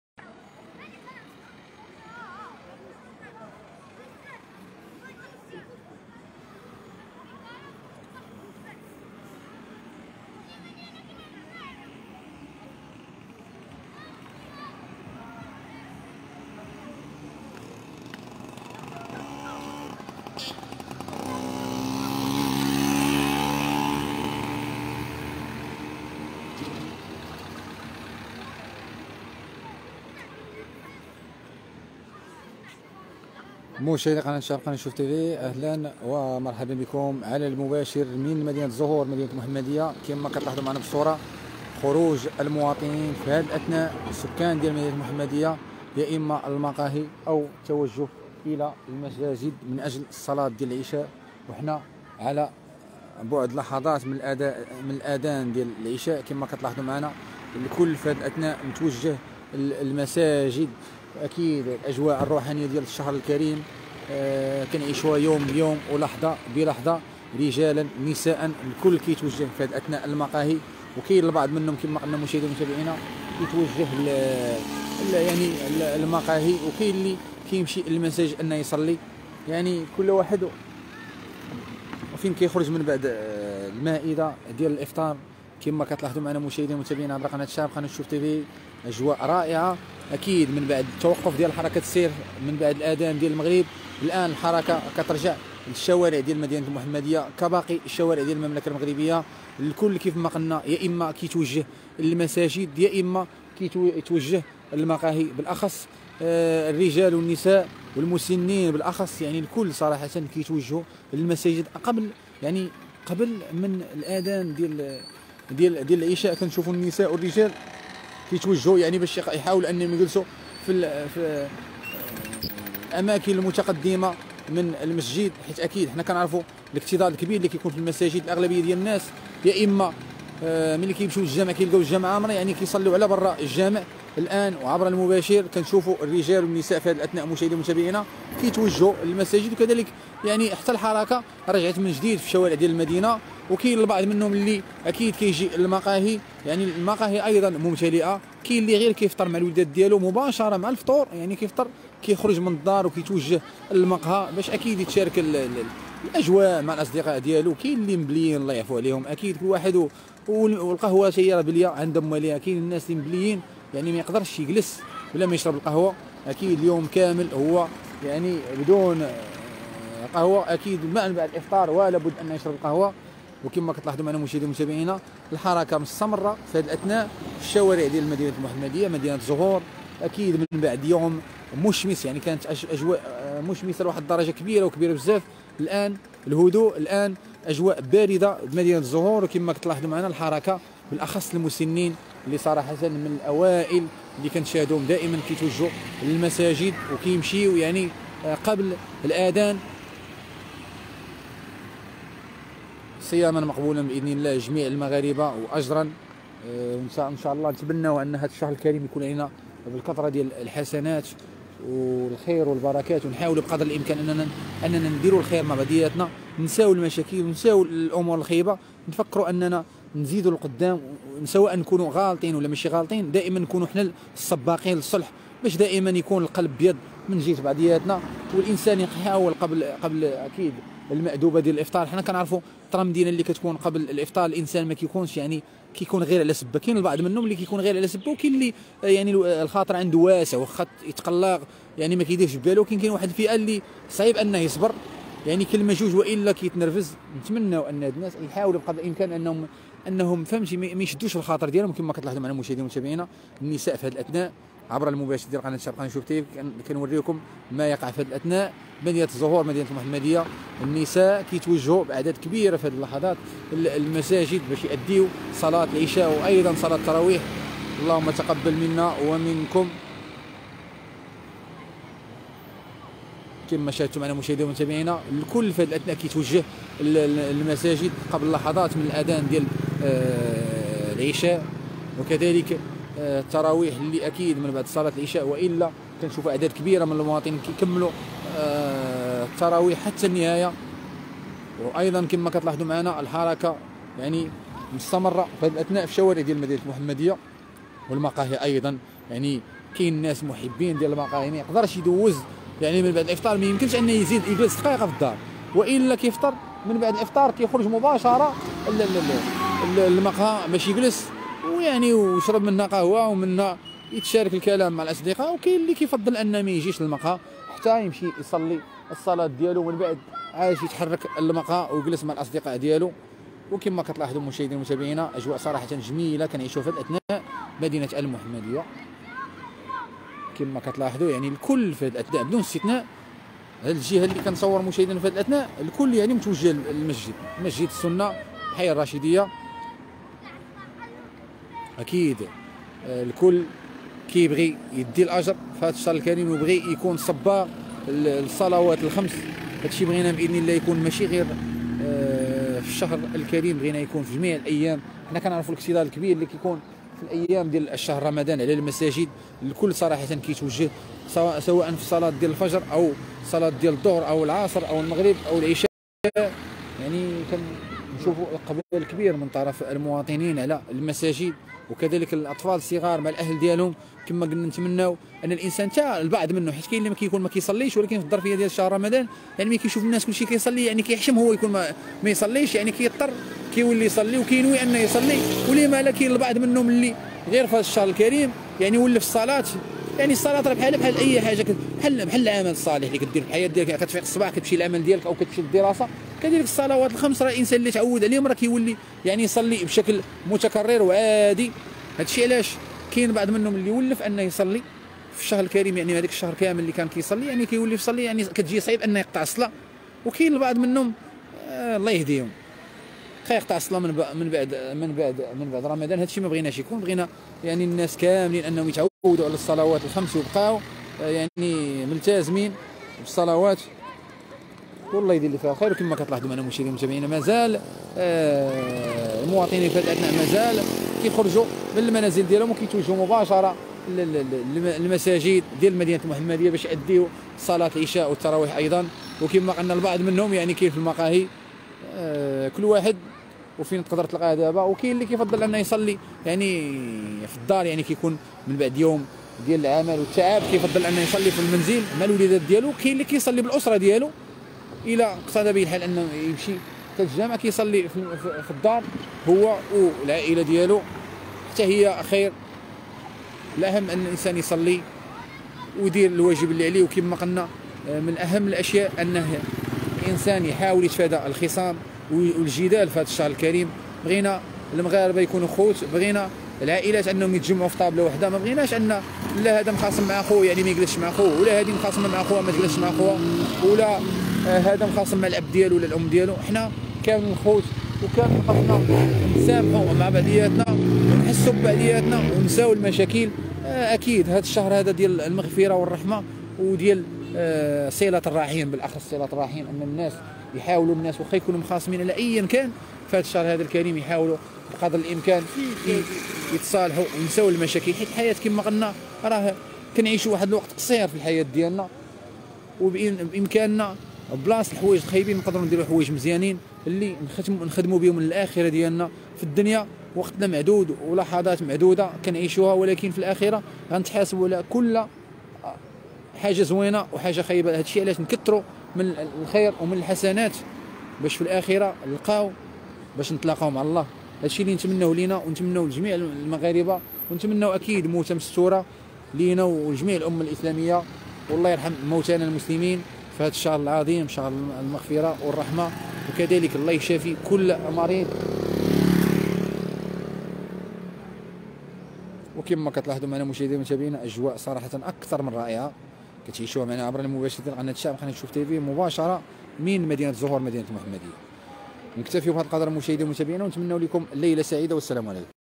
ساكنة المحمدية خارجة بعد الفطور للمقاهي وكاين لي في اتجاه المساجد لآداء صلاة العشاء والتراويح
على المباشر:ساكنة المحمدية خارجة بعد الفطور للمقاهي وكاين لي في اتجاه المساجد لآداء صلاة العشاء والتراويح يوم 16 رمضان